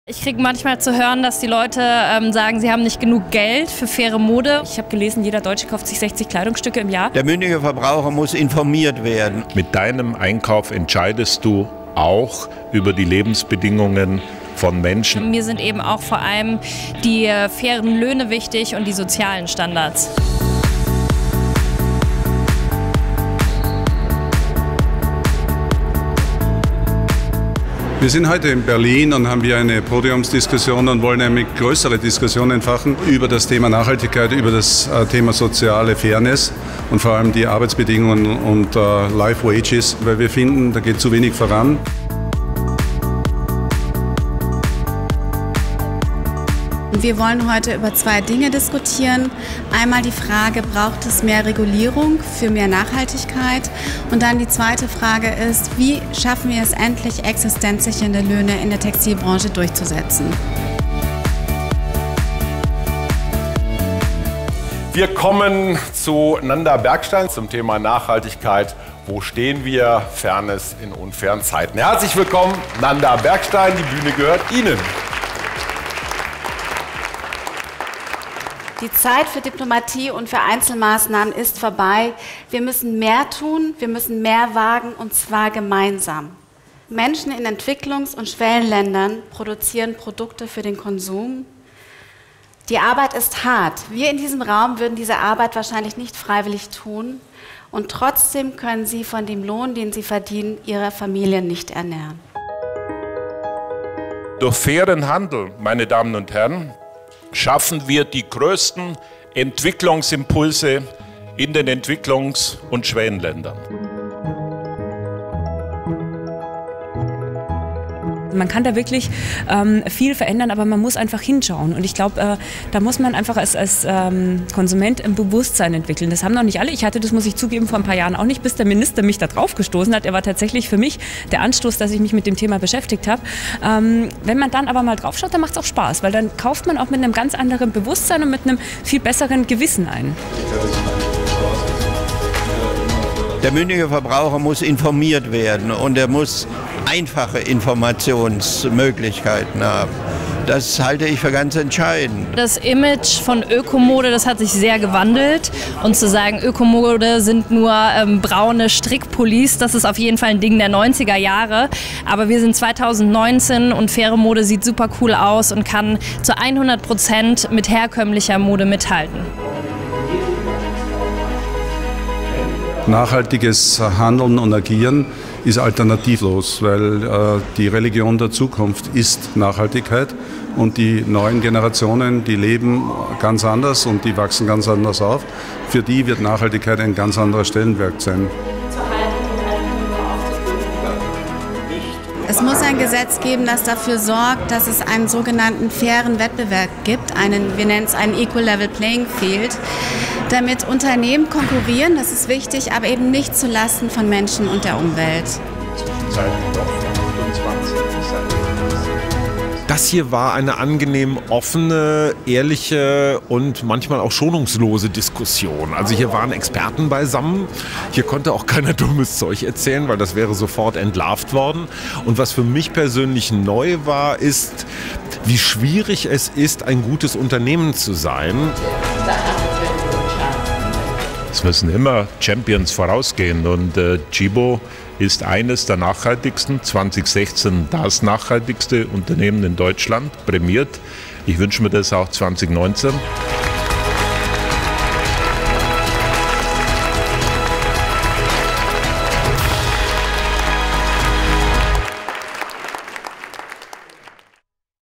Tchibo Event: Das Ende der Nachhaltigkeit?
Nachhaltigkeit wird immer mehr zum Mainstream. Gleichzeitig sind die Zustände in der Welt nicht fair. Warum etwa werden noch immer keine existenzsichernden Löhne in der Textilindustrie gezahlt? Tchibo diskutierte dazu am 3. April 2019 in Berlin mit Bundesminister Dr. Gerd Müller, Ex-Vizekanzler Joschka Fischer, Tchibo CEO Thomas Linemayr, sowie u.a. den Models und Nachhaltigkeitsbotschafterinnen Barbara Meier und Marie Nasemann.